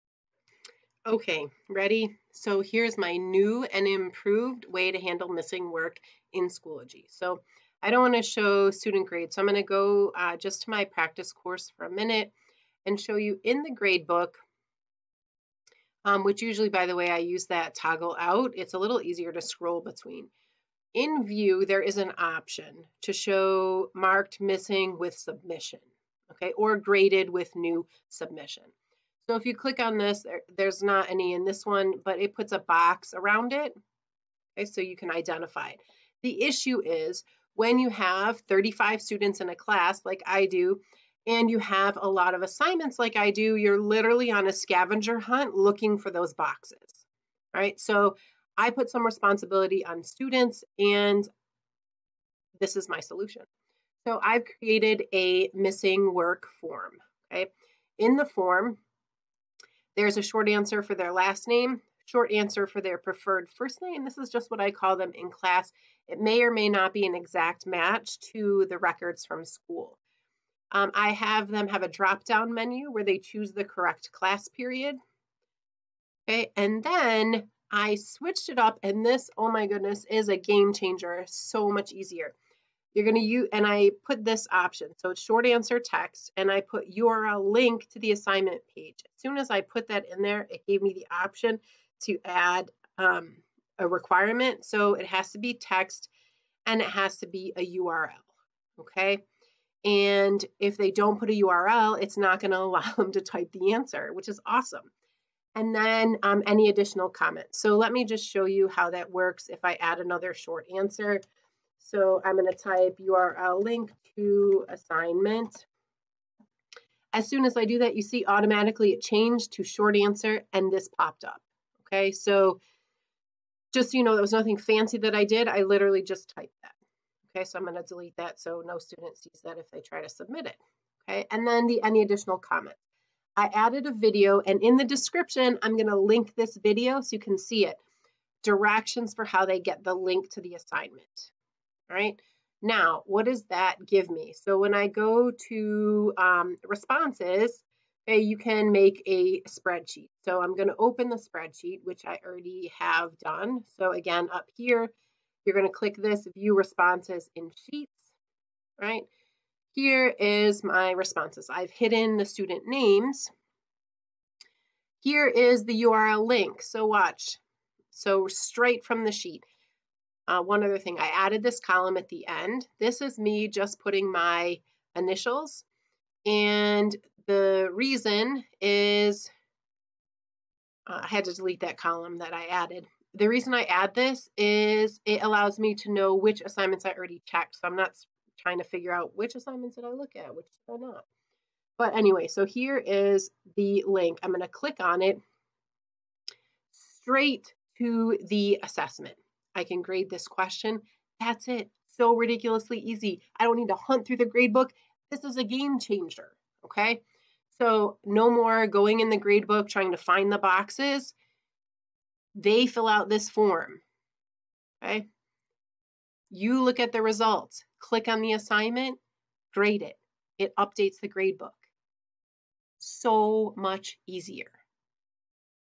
Schoology: Managing Missing (or resubmitted) Work the Easiest Way! It's a GAME CHANGER!
This video shows literally the best way to manage missing and resubmitted work. It's literally my favorite solution! You can check missing work so simply this way!